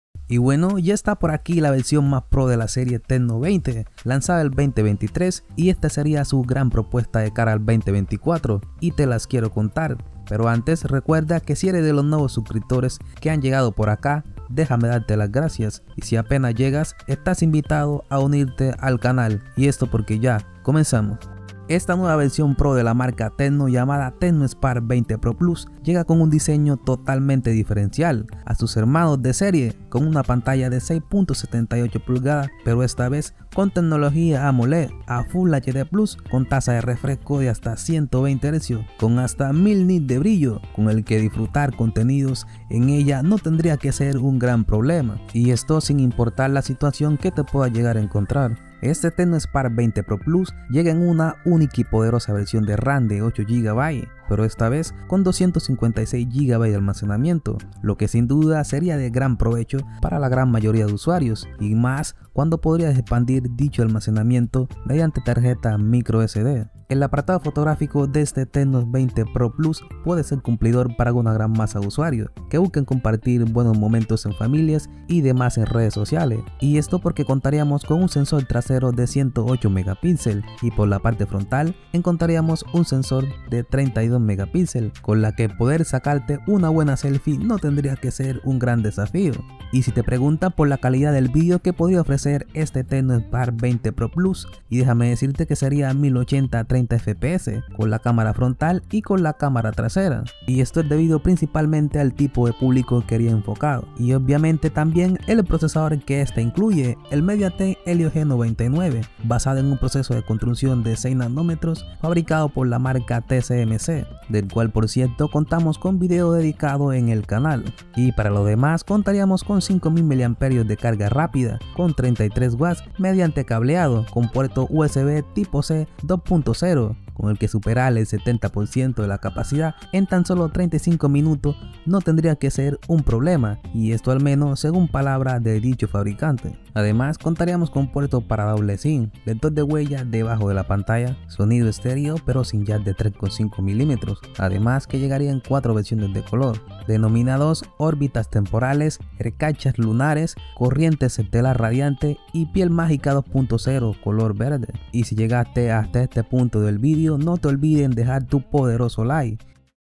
Tecno Spark 20 PRO PLUS | Te lo Recomiendo 🤔
El integrante mayor de la serie Tecno Spark 20 es oficial de cara al 2024 con una propuesta diferente al del resto de sus hermanos de serie dejando muy buen sabor de boca.